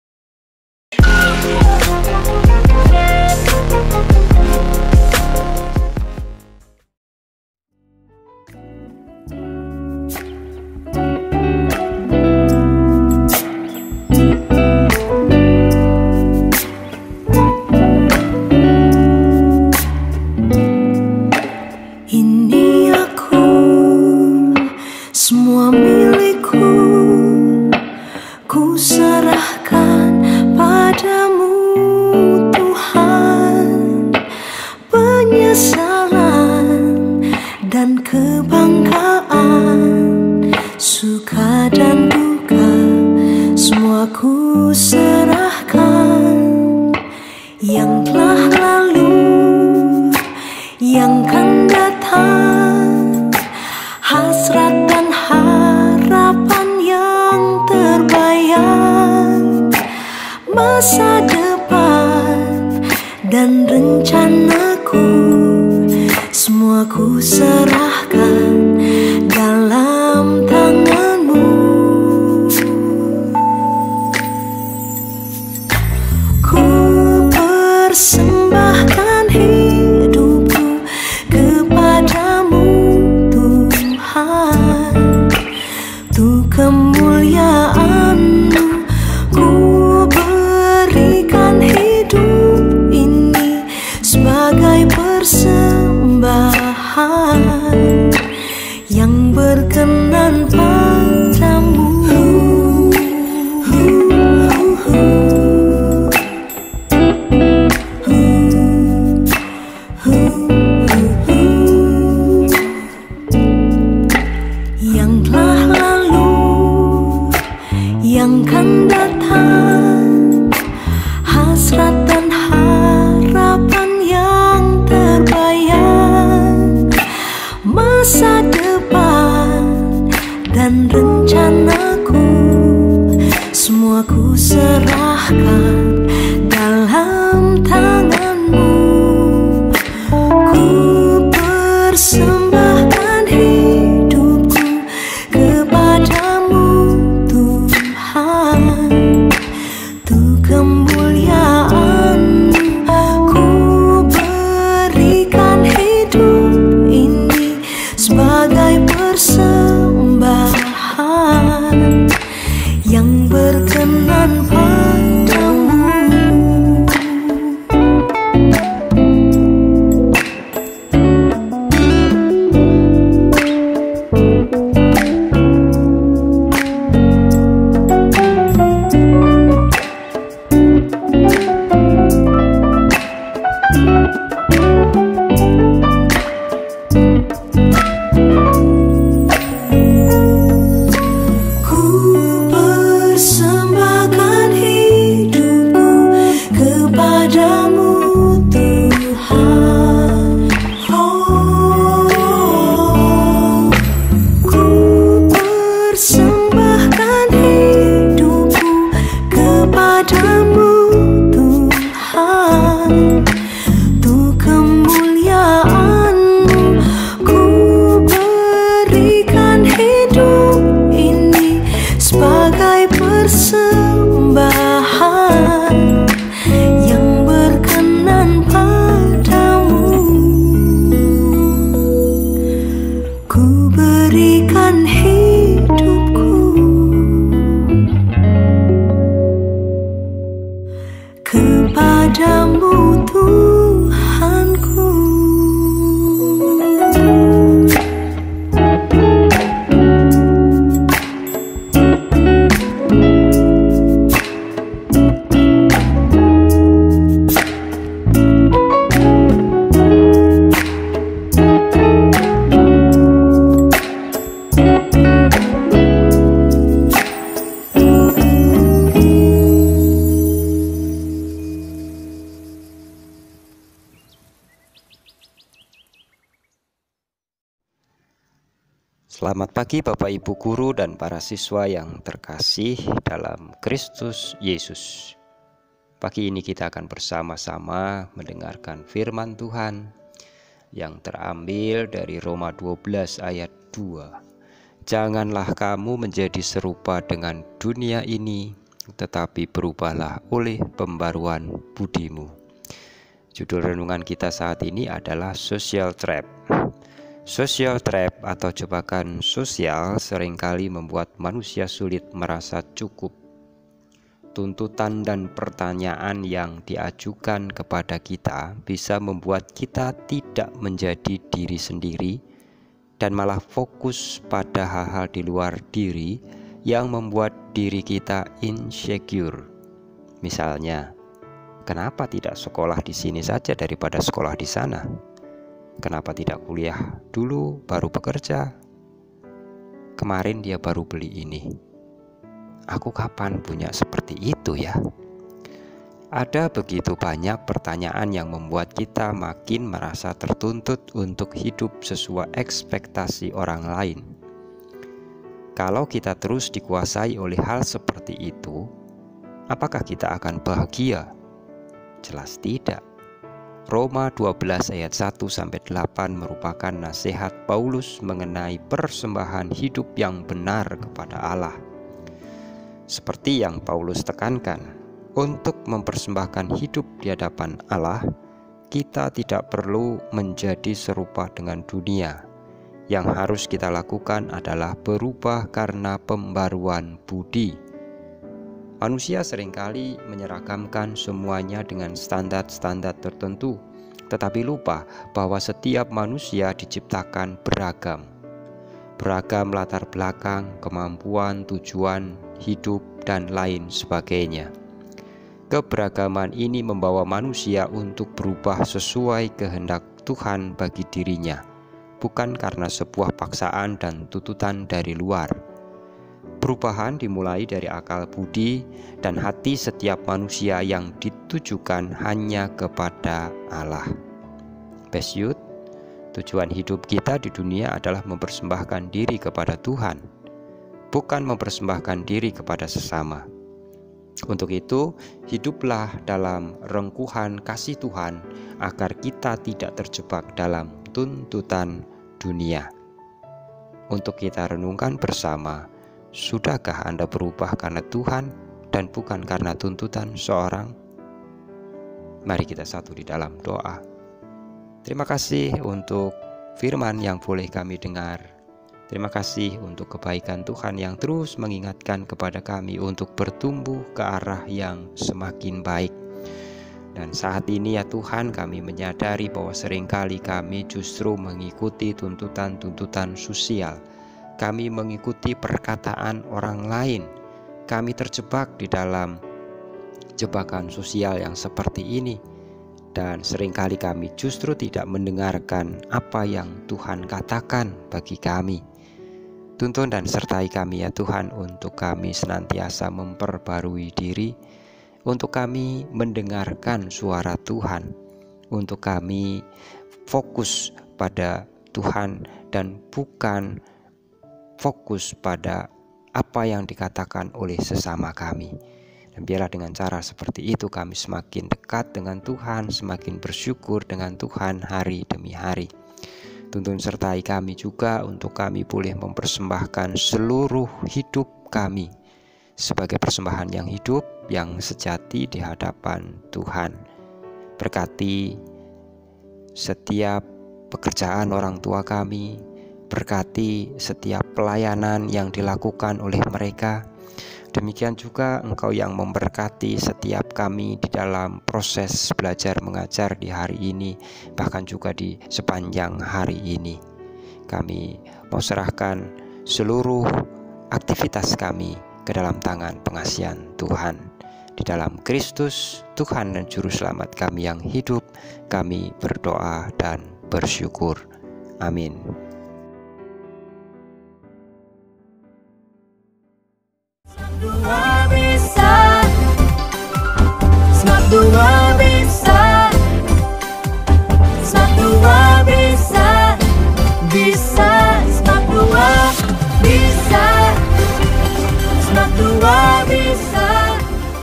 Renungan Siswa | SOCIAL TRAP | 21 Januari 2022
Renungan Siswa, 21 Januari 2022

Social Trap

Roma 12:2
Janganlah kamu menjadi serupa dengan dunia ini, tetapi berubahlah oleh pembaharuan budimu